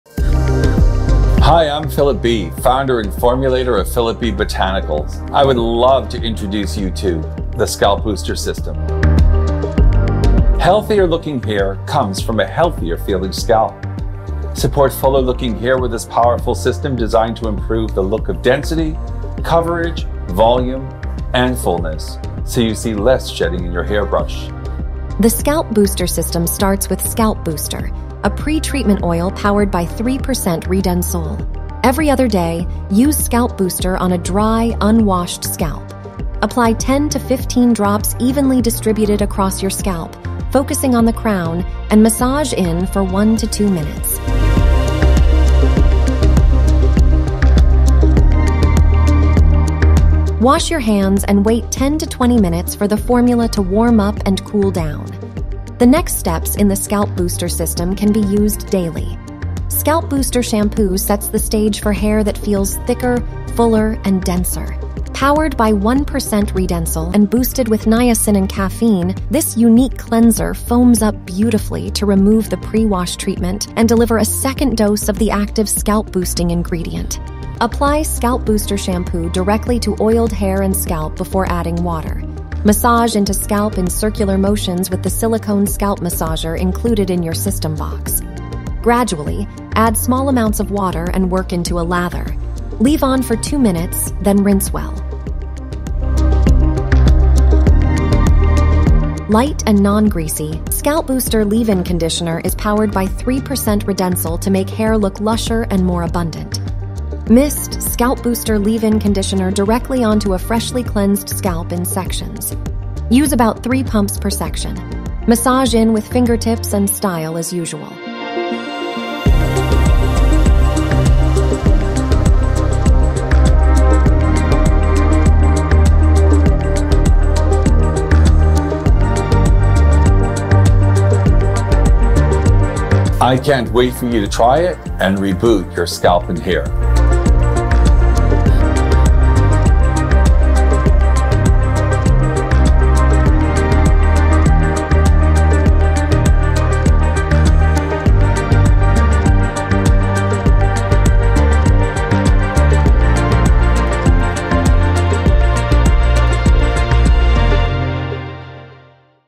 Scalp Booster System
Healthy-looking hair comes from a healthier-feeling scalp. Support fuller looking hair with this powerful system. It’s designed to improve the look of density, coverage, volume and fullness, so you see less shedding in your hair brush.

It starts with the Scalp Booster Pre-Wash, powered by 3% Redensyl.® In clinical trials of this ingredient, 71% of participants saw their hair take on a thicker look and feel after just three months of use.

What makes Redensyl® even more effective is what we’ve paired it with: Antioxidant green tea. A plant-based compound designed to create a warming sensation as part of the formula’s delivery system. Plus a mix of salicylic acid (an exfoliant) and citrus extracts that help solubilize sebum, breaking down the barrier coating on the scalp for more effective distribution.

The Scalp Booster Shampoo bolsters the effect on hair: Made with Redensyl®,  niacin and caffeine, it sets the stage for hair that feels thicker, fuller and denser. Keratin amino acids condition, while green tea and a natural wood extract serve as nourishing antioxidants.

Light and non-greasy, the Scalp Booster Leave-In Conditioner helps extend the benefits even further. It’s powered by 3% Redensyl® to make hair feel more lush and abundant. Salicyclic acid, a natural exfoliant, to make your scalp feel healthier. A blend of plant-derived extracts and potent wheat and rice amino acids to condition. Even better, we’ve encapsulated all the active ingredients in time-released Safflower Oleosomes — an advanced botanical delivery system to keep your hair and scalp feeling and looking their best.

Use our Silicone Scalp Massager in small circular motions while shampooing to support healthy scalp and hair. The ergonomic handle fits comfortably in the hand; gentle cone-shaped silicone tips help ensure comfort.